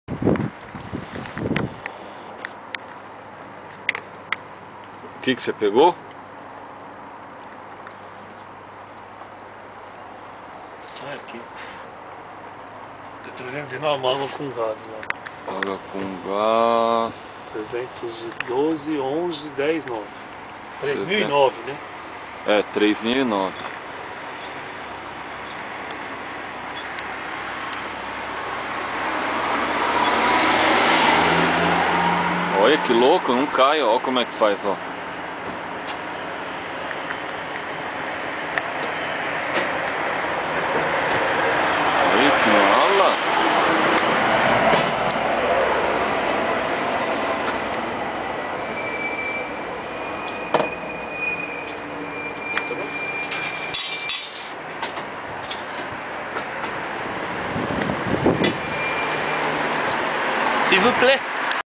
Loja de Conveniência em Strasburg França
Conveniência em Strasburg França, muito loko , como é facil comprar uma água, hehehehehehe